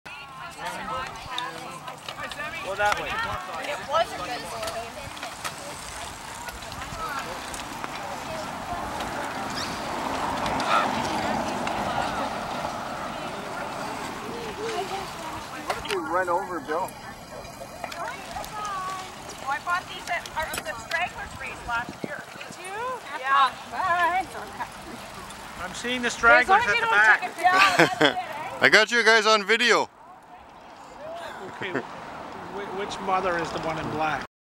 TSR Dryland biking 2012
Timmins Ski Racers start Dryland training for the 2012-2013 season biking from Gilles Lake (Sept.2012).